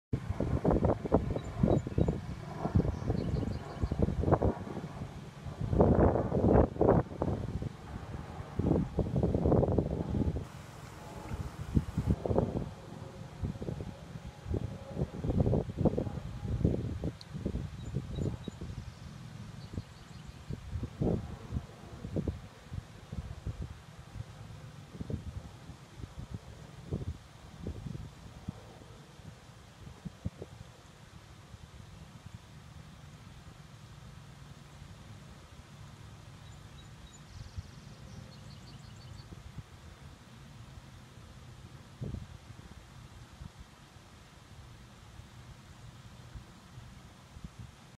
El ánade real o azulón (Mallard)
Uno de los patos más comunes, está pareja de patos fue captada en un creak en el estado de Virginia, estados unidos!!!